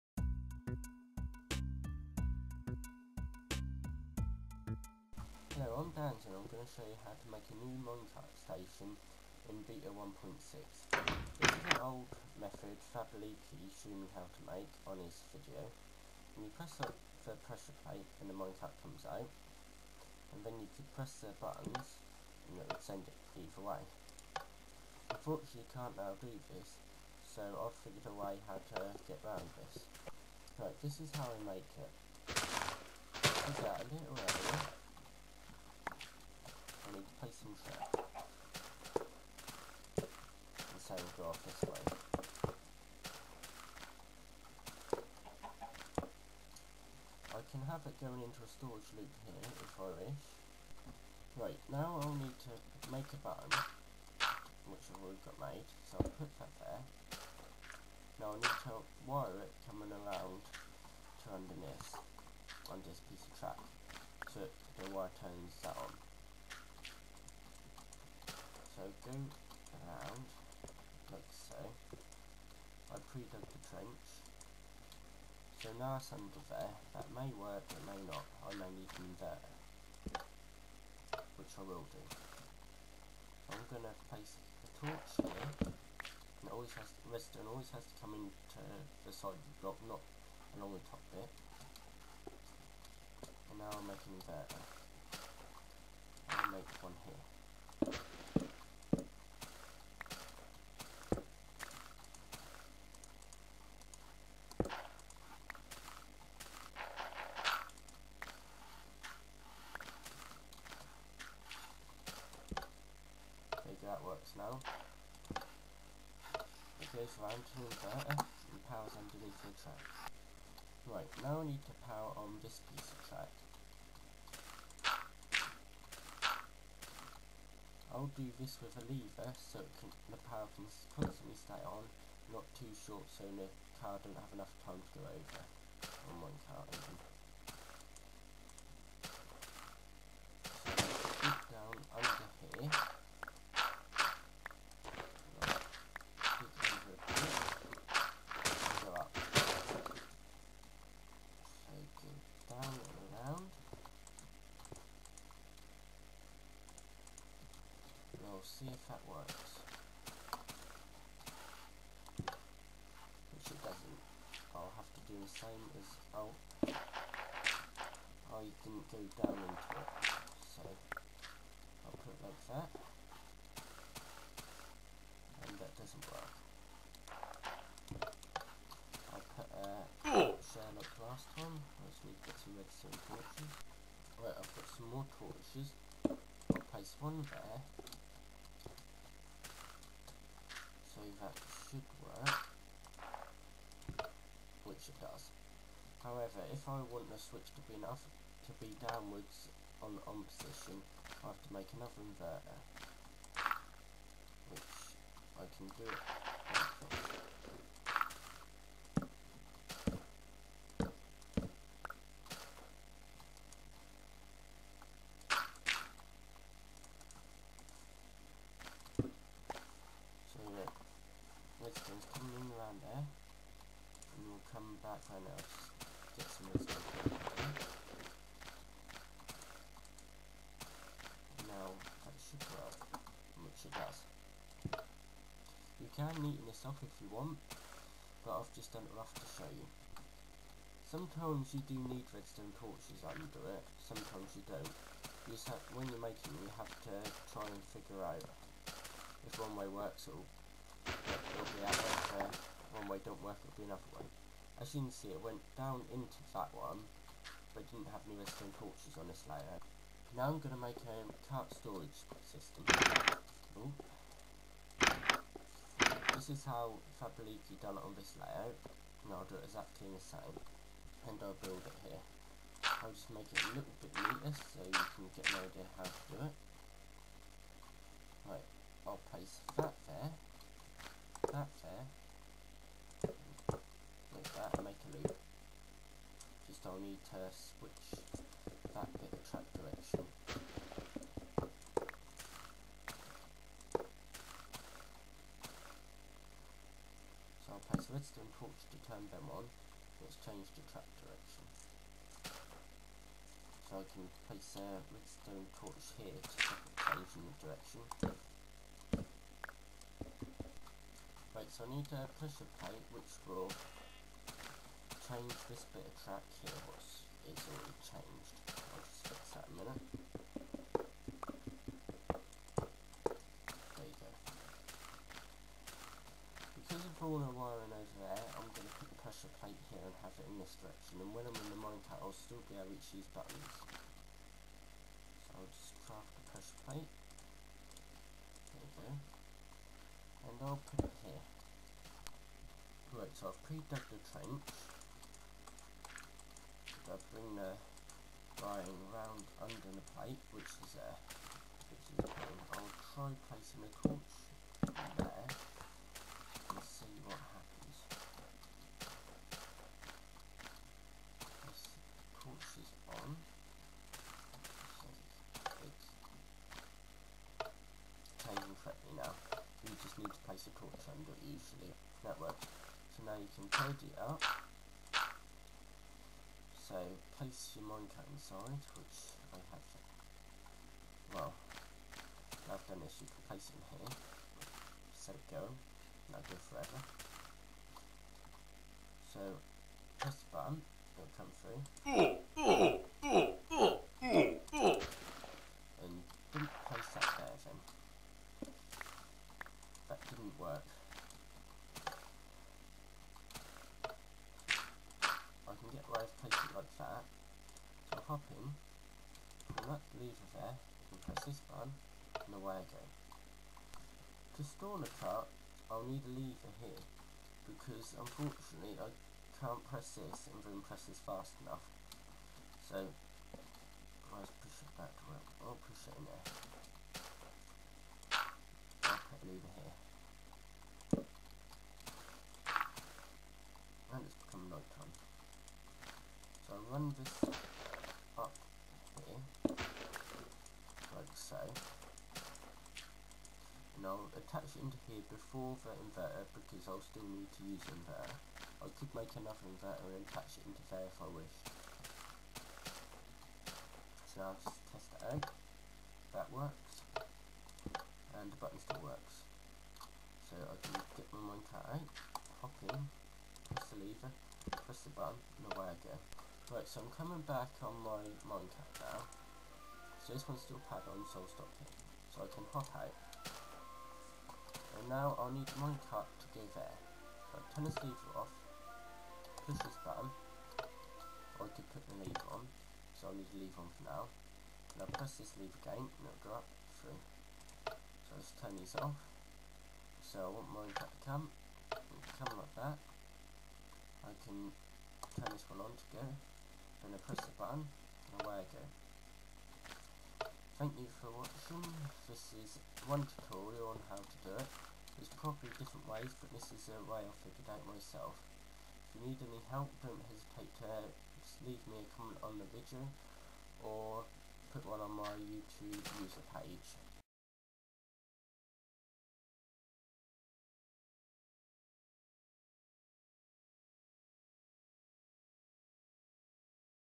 Minecart Station Beta 1.6
This is how you can make a minecart station in beta 1.6, Enjoy!